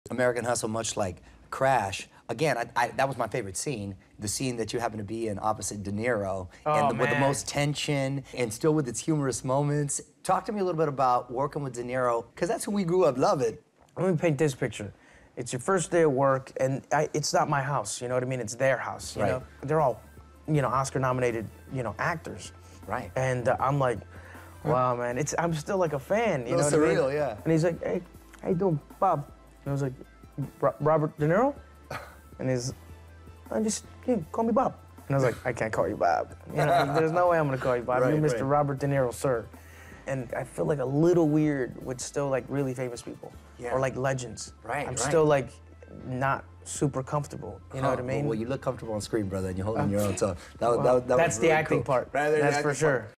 Michael Peña Talks Working With Robert De Niro | Mario Lopez: One On One
Michael Peña talks about his excitement on getting to work with Robert De Niro.

Fresh off American Hustle and Cesar Chavez, actor Michael Peña is quickly becoming Hollywood royalty. Find out about his humble beginnings and how he went from high school dropout to master thespian.